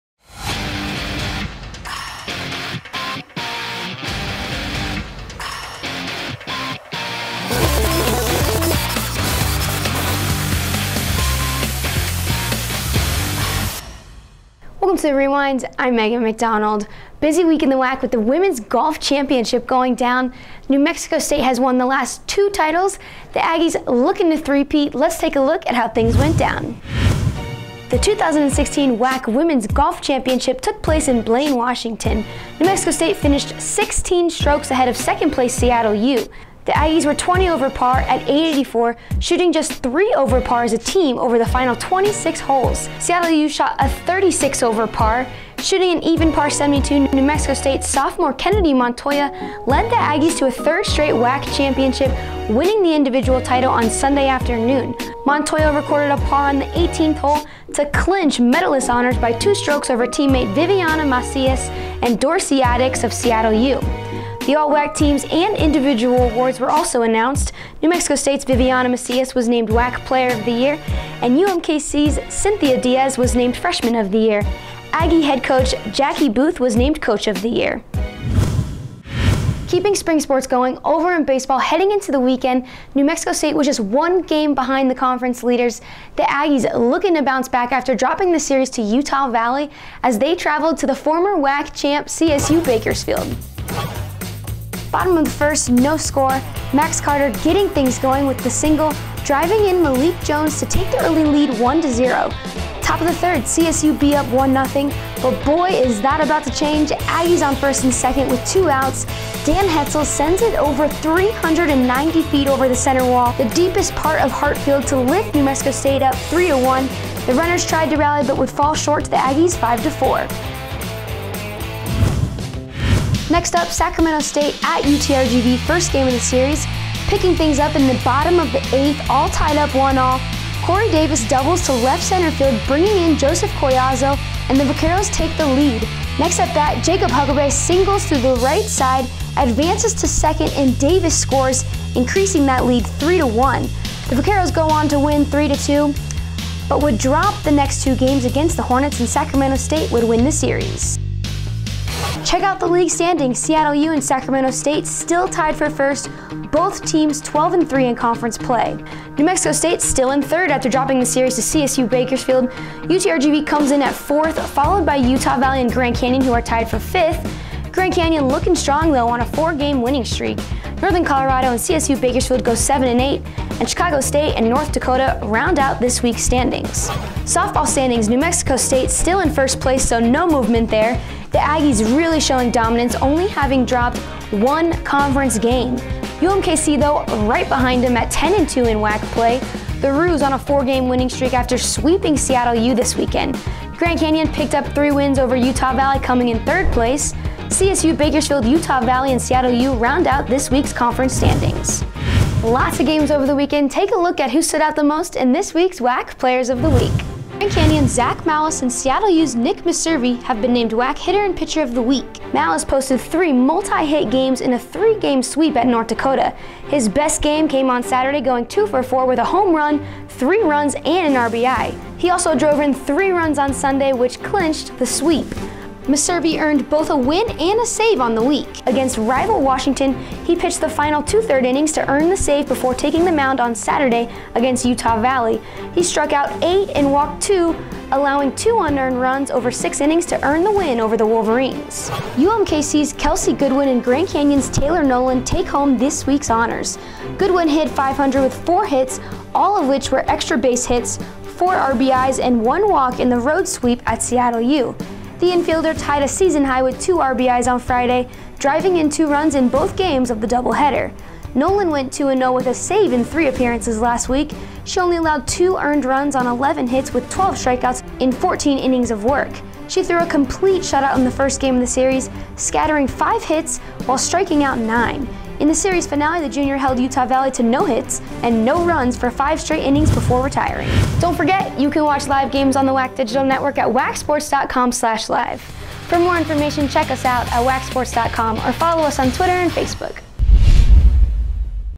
WAC Digital Network: The Rewind - April 25
WAC Digital Network host Megan McDonald recaps the week in the WAC with women's golf, NM State vs CSU Bakersfield baseball, UTRGV vs Sac State baseball, league standings and WAC Players of the Week.